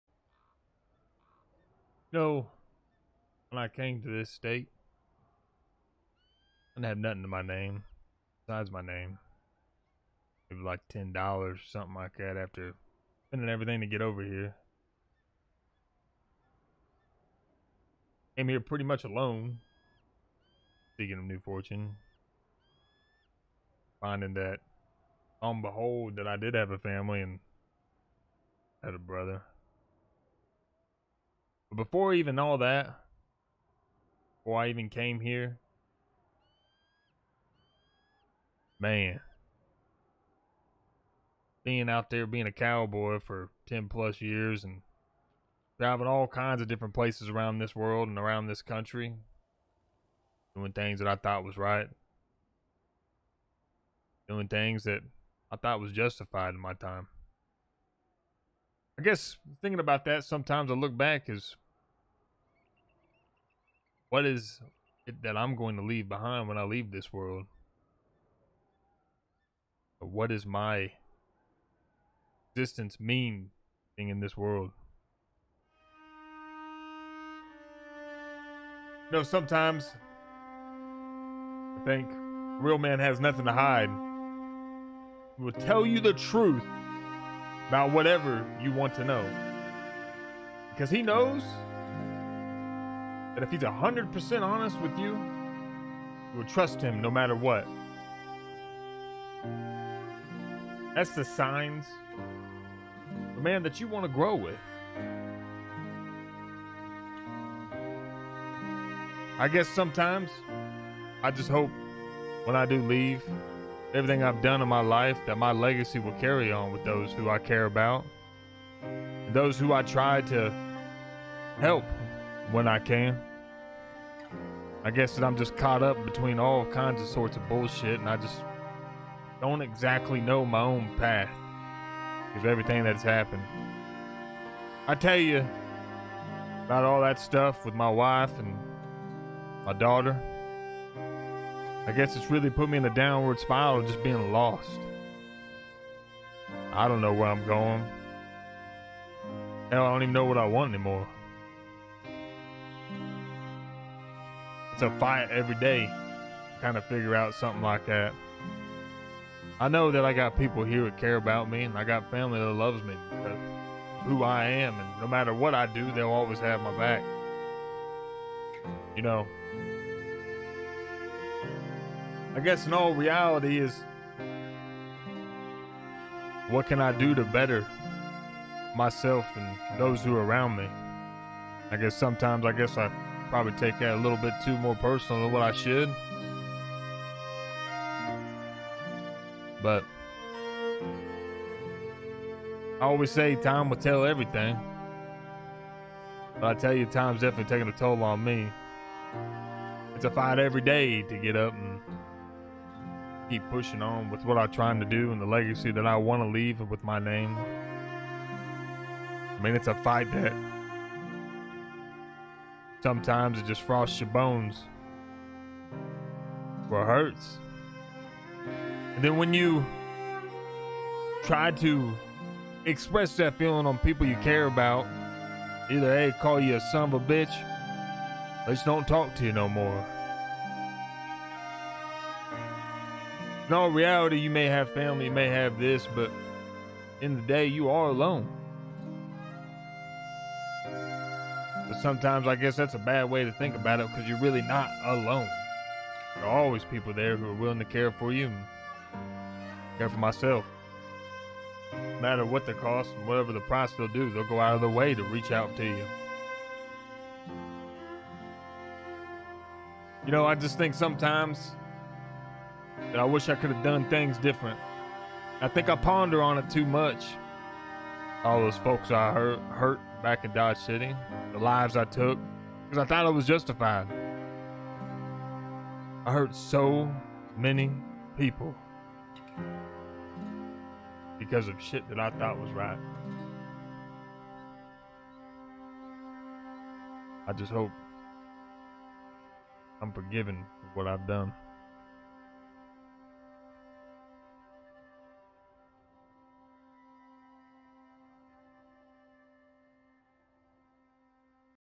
Johns Journal | V.3. | Wild West Roleplay
As more John's Back story unfolds. He Often does alot of Reflecting and You see his way of Thinking

 🔥Social Medias 🔥

 TikTok.com/Goeke762